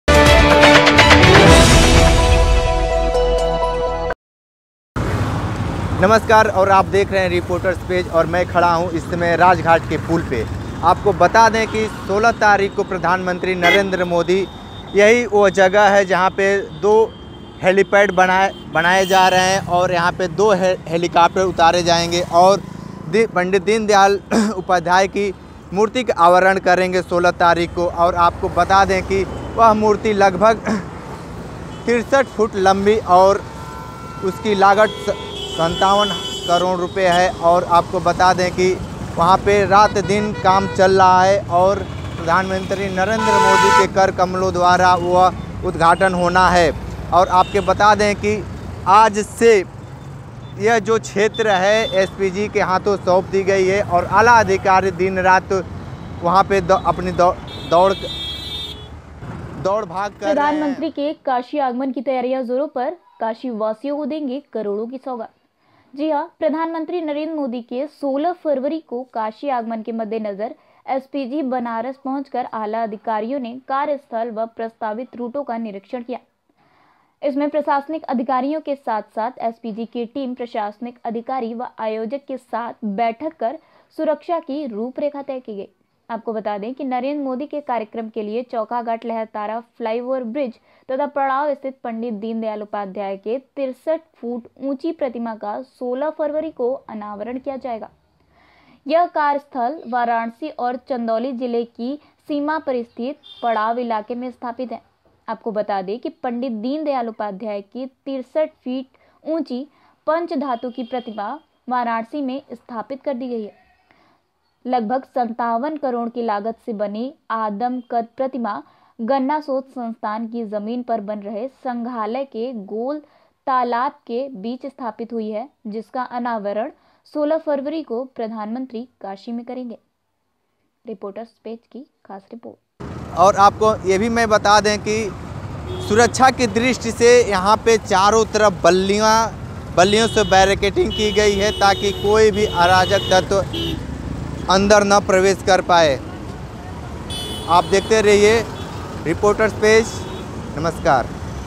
काशी आगमन पर तैयारियां जोरो पर पीएम नरेंद्र मोदी देगे करोड़ों का सौगात
वाराणसी - काशी आगमन पर तैयारियां जोरो पर पीएम नरेंद्र मोदी देगे करोड़ों का सौगात।
पीएम नरेंद्र मोदी के 16 फरवरी के काशी आगमन के मद्देनजर बुधवार को एसपीजी बनारस पहुंच कर दोपहर में आला अधिकारियों का जत्था। कार्यस्थल व प्रस्तावित रूटों का निरीक्षण किया। यह प्रशासनिक अधिकारियों के साथ एसपीजी की टीम प्रशासनिक अधिकारियों व आयोजकों के साथ। बैठक कर सुरक्षा की रूपरेखा तय किया। नरेंद्र मोदी के कार्यक्रम के लिए। चौकाघाट लहरतारा, फ्लाईओवर ब्रिज तथा पड़ाव स्थित पंडित दीनदयाल उपाध्याय के 63 फुट ऊंची प्रतिमा  का 16 फरवरी को आवरण होगा। यह स्मार्कस्थल
वाराणसी और चंदौली जिले की सीमा पर स्थित पड़ाव इलाके में स्थापित है  पंडित दीनदयाल उपाध्‍याय की 63 फीट ऊंची पंच धातु की प्रतिमा वाराणसी में स्थापित।  6.21 करोड़ की लागत से बनी आदमकद प्रतिमा गन्‍ना शोध संस्‍थान की जमीन पर बन रहे संग्रहालय में गोल तालाब के बीच स्‍थापित हुई हैं